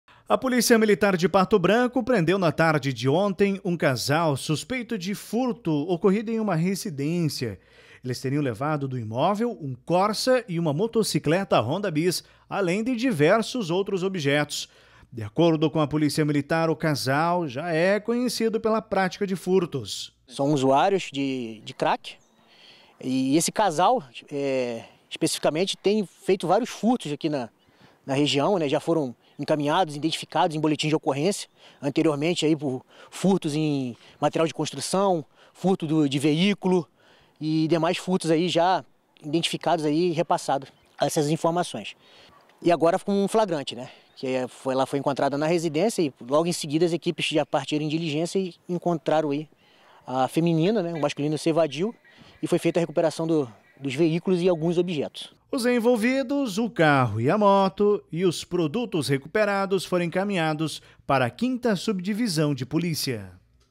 Casal é preso em Pato Branco após furtarem dois veículos
Segurança: Além de diversos outros bens de uma residência.